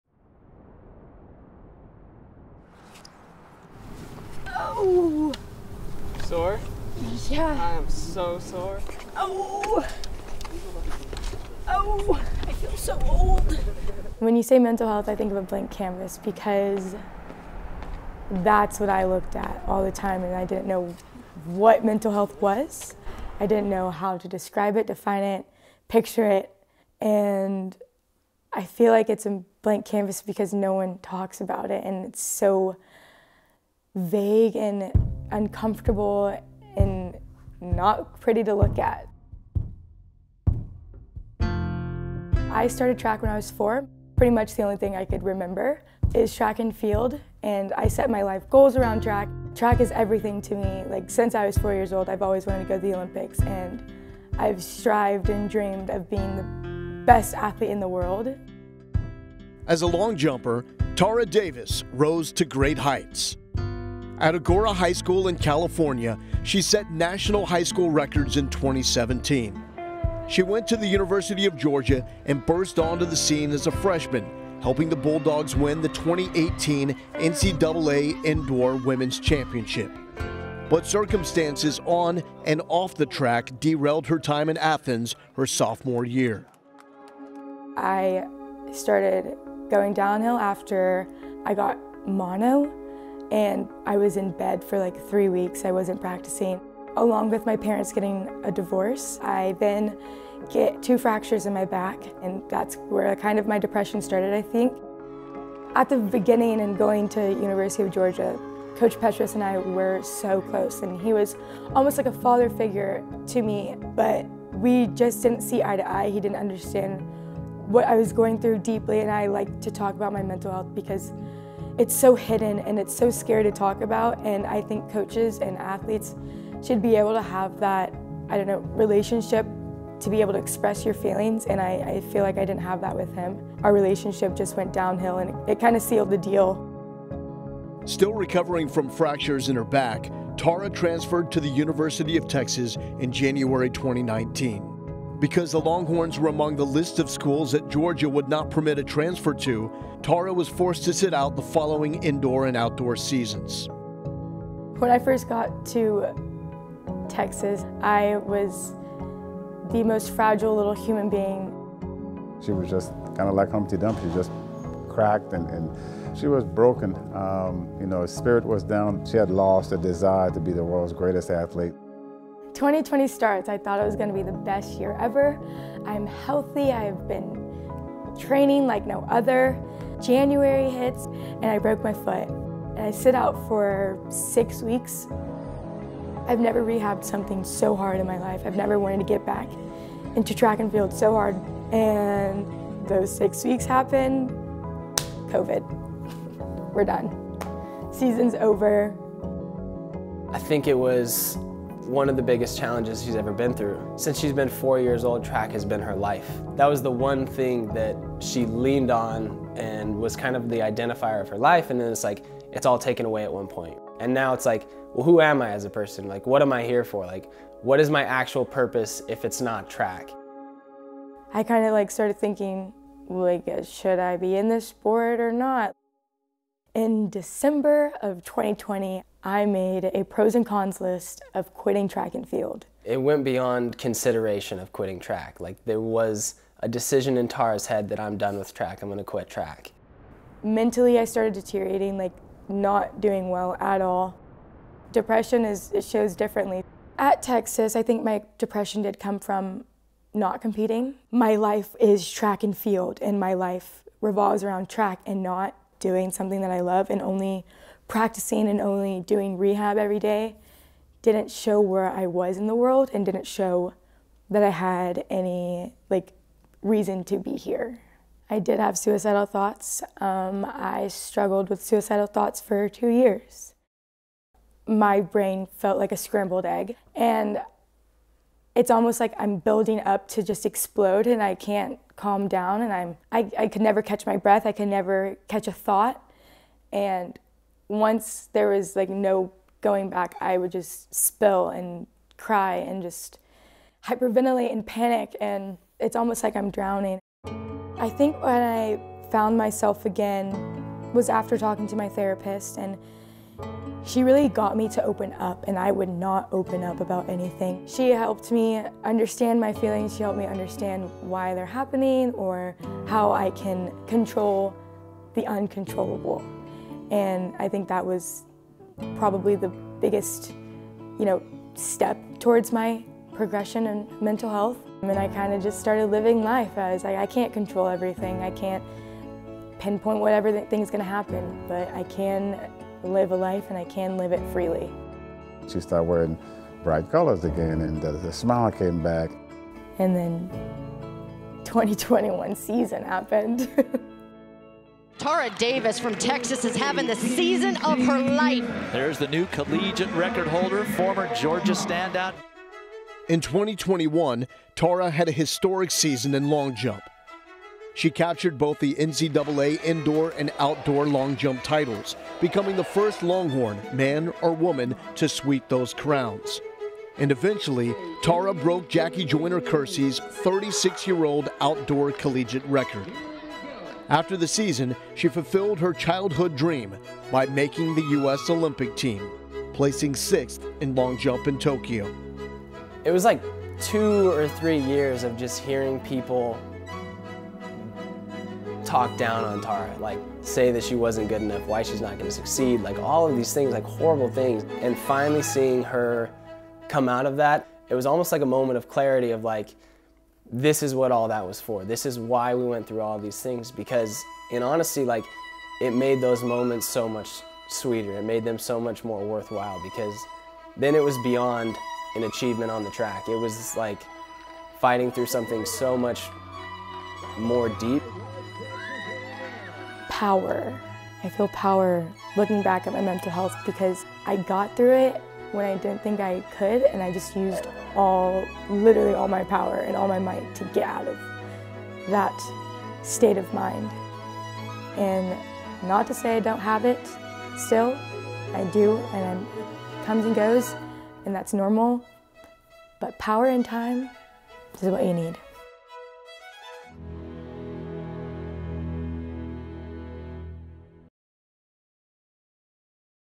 Opening up: How Tara Davis found herself | Longhorn Network
Olympian Tara Davis shares how she overcame struggles to reach new heights on and off the track.